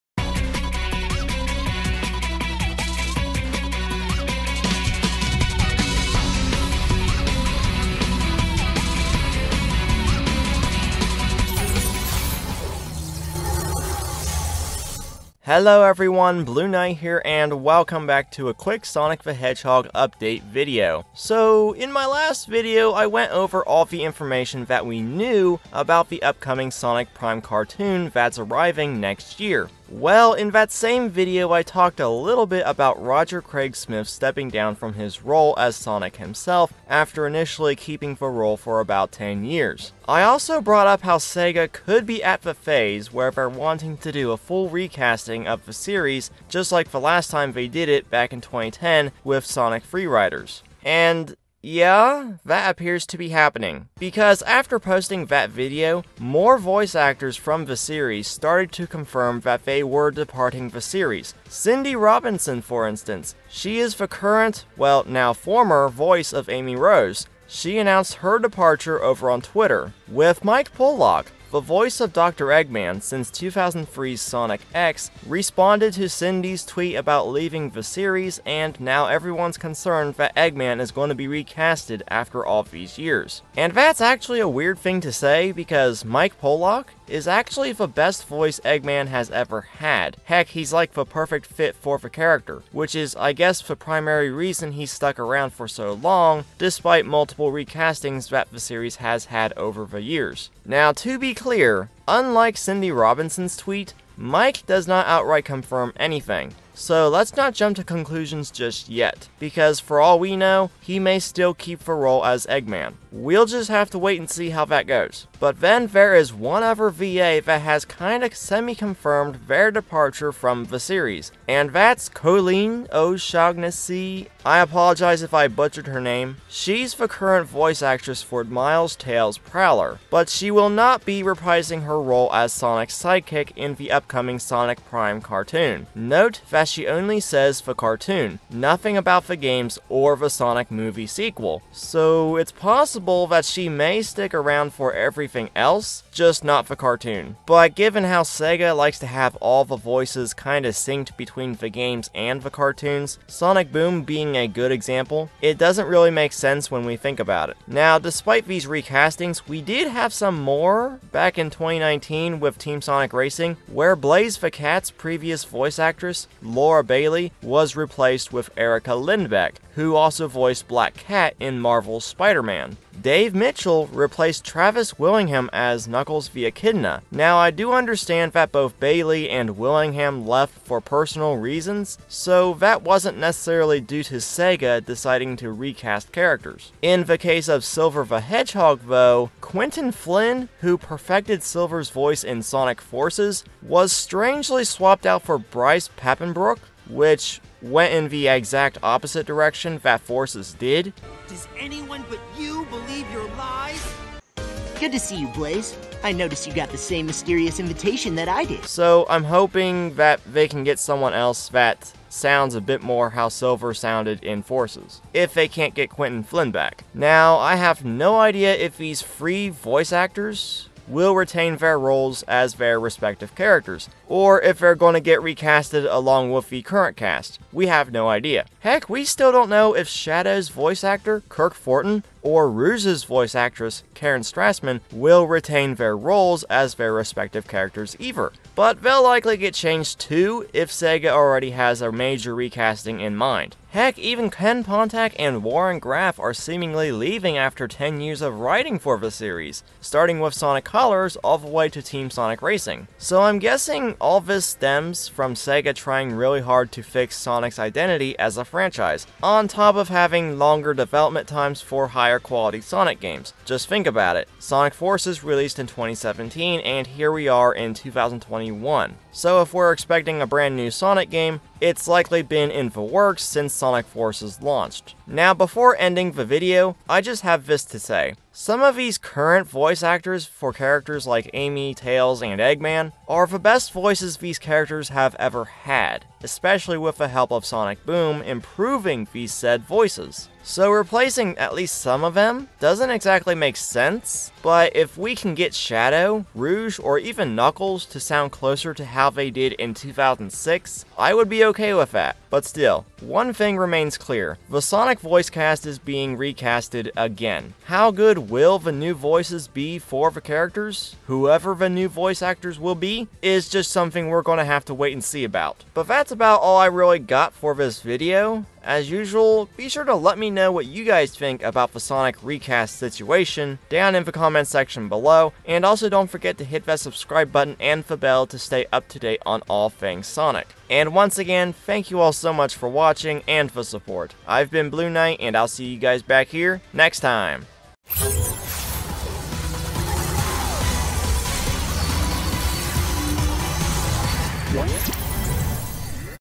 Sonic The Hedgehog - Major Recast Is Happening!? - Both Voice Actors & Writers Departing!?
So in the past few days following the announcement that Roger Craig Smith left his role as Sonic & the reveal of Sonic Prime. Multiple voice actors from the Sonic series have been confirmed to be leaving the series, even the writers, Pontaff.  Let's Check It Out!

So what do you guys think about this? You guys can also find me over at these places